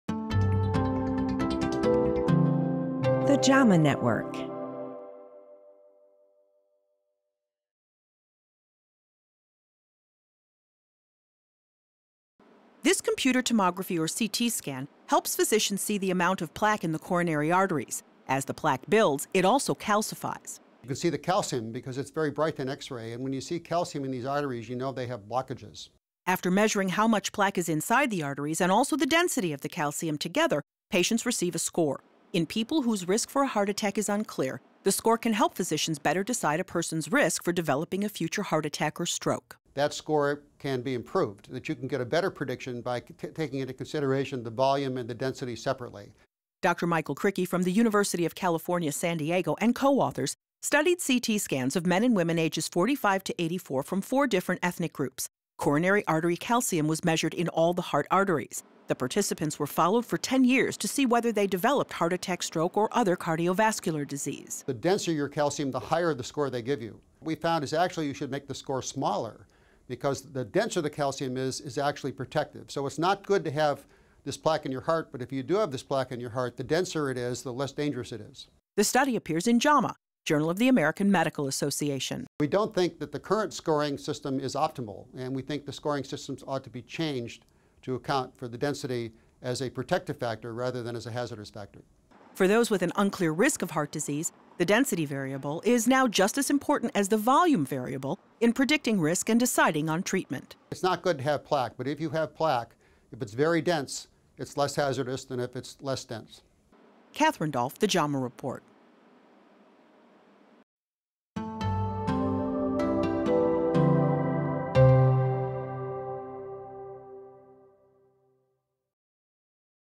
DENSITY OF CORONARY ARTERY CALCIUM MAY HELP PREDICT RISK OF HEART ATTACK AND STROKE
Calcium plaque in the arteries of the heart is a sign of changes that can lead to heart attacks. To measure this calcium physicians sometimes use a form of CAT scan which provides information about how much calcium is present and how dense it is. A new study examined whether measuring calcium amount and density separately might better help predict if a person is at risk for developing a heart attack or stroke. Catherine Dolf has more in this week's JAMA Report.